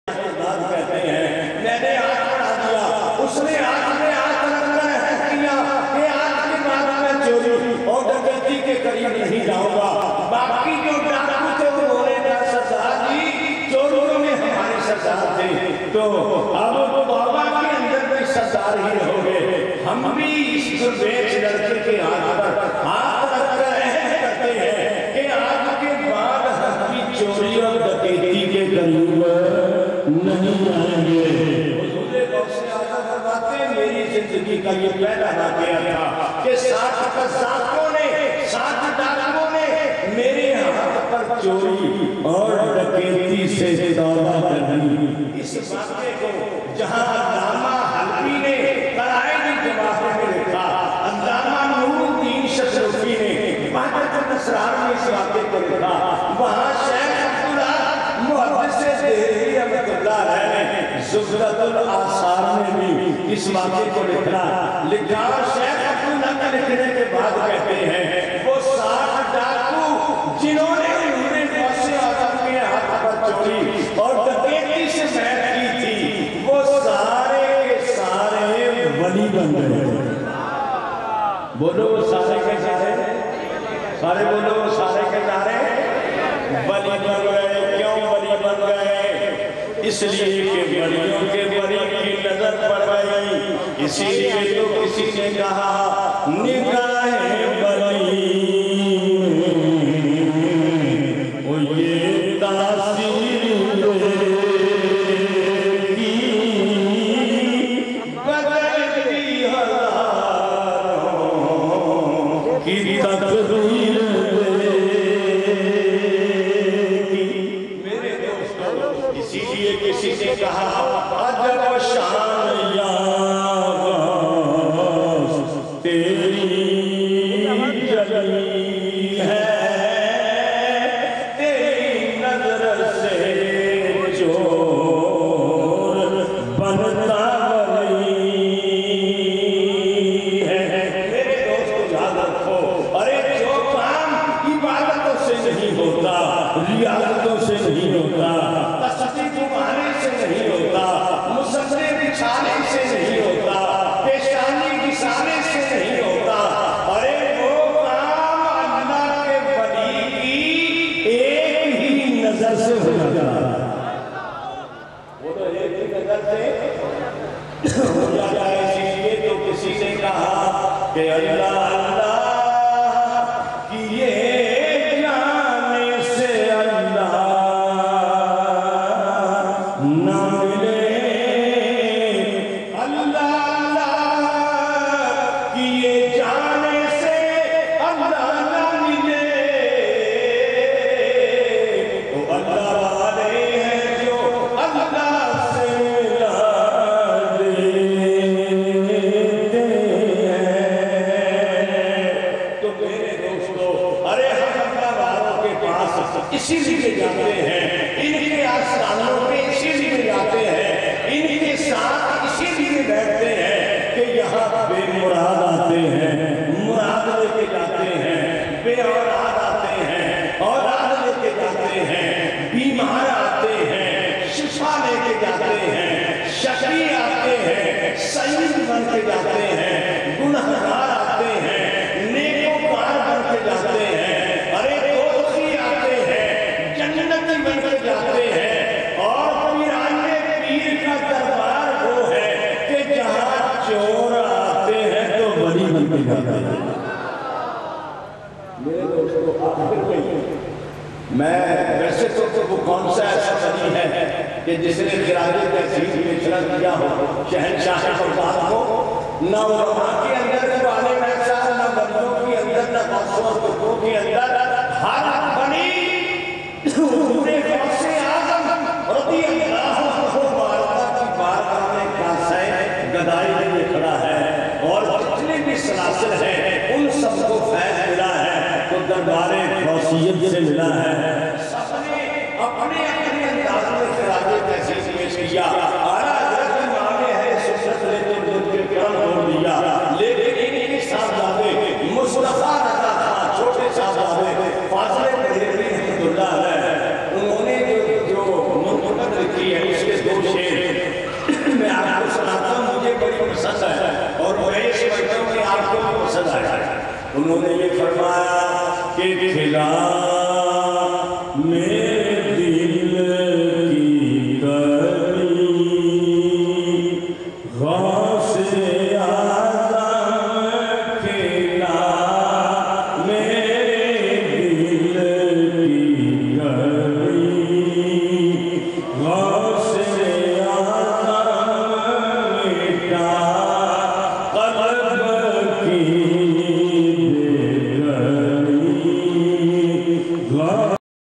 BARI GYARHWIN BARE PIR KI 10
Al-Sheikh Hashimuddin Al-Gaylani in "Bari Gyarhwin Bare Pir ki" at Karachi on 06th April 2009